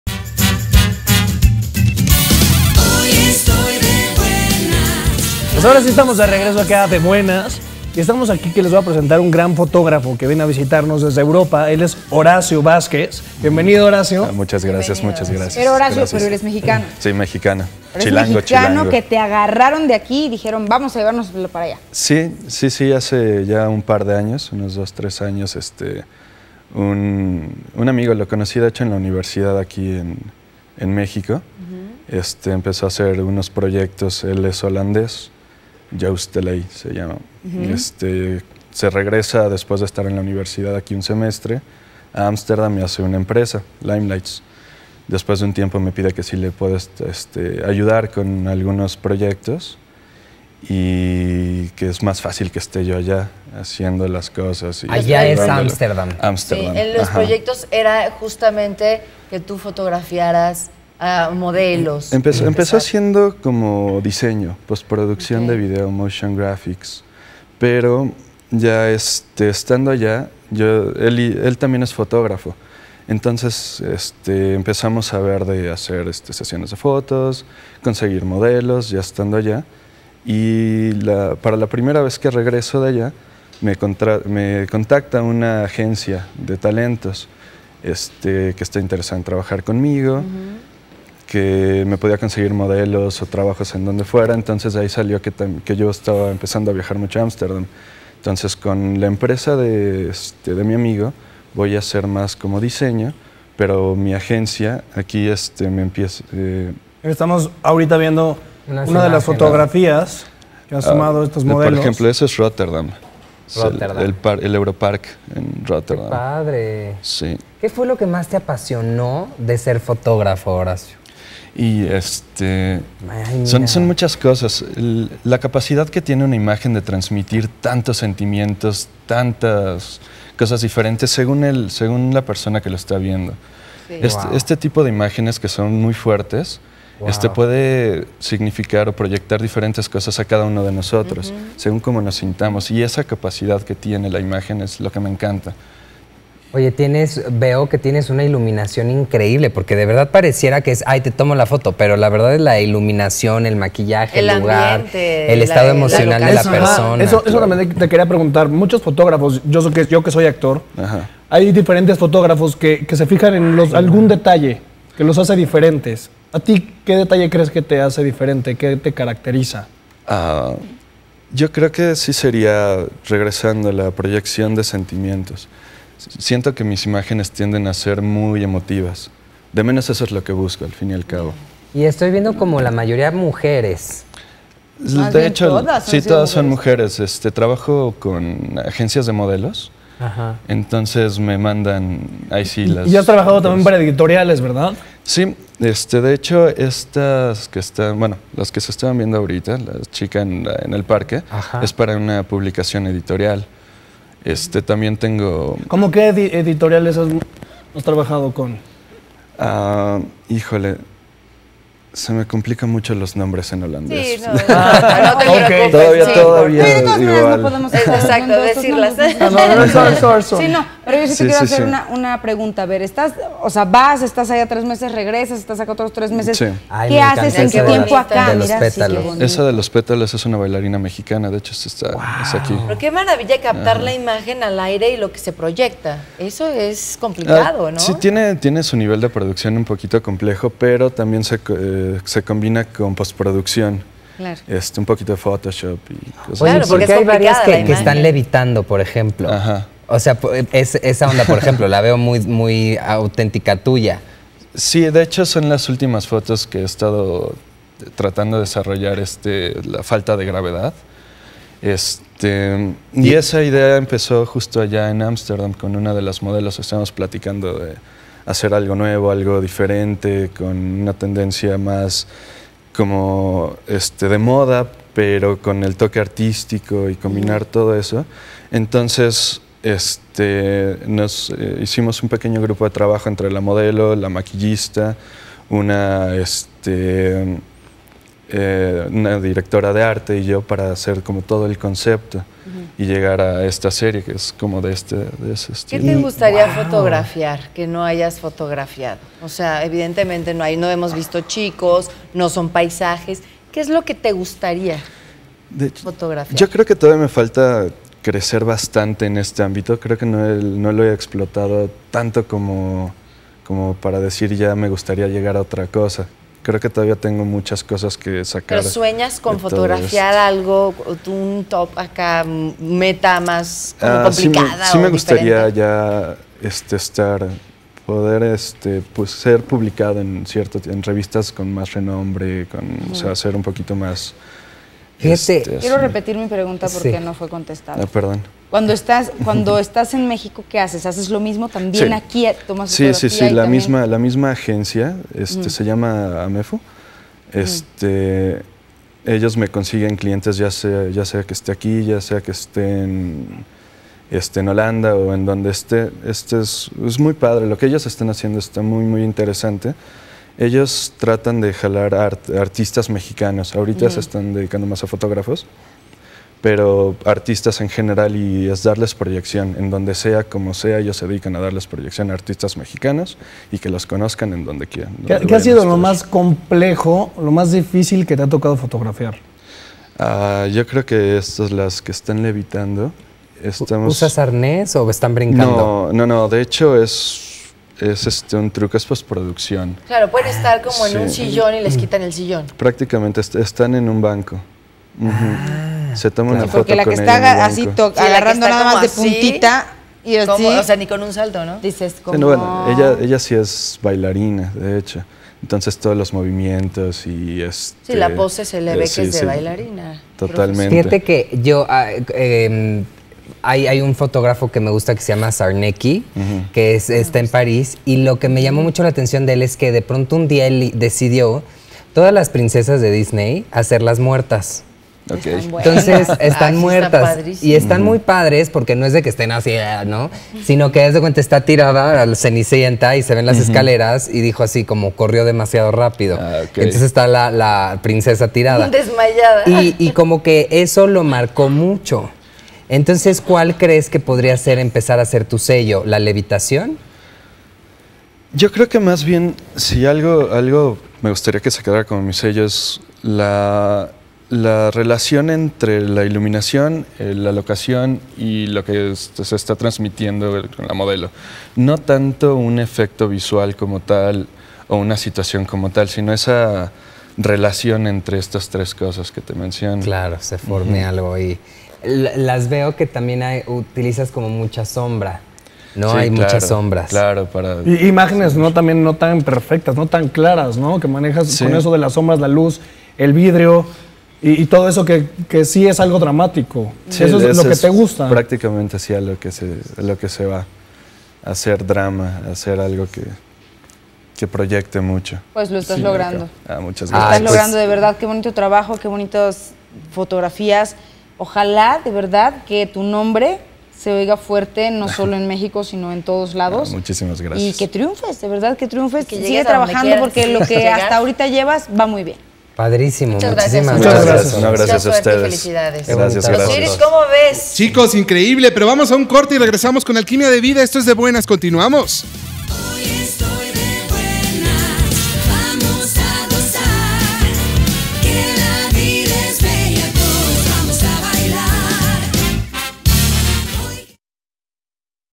Fotografía Editorial. Horacio Vásquez
Nuestro querido fotógrafo Horacio Vásquez nos habla de su pasión y experiencia por la fotografía editorial.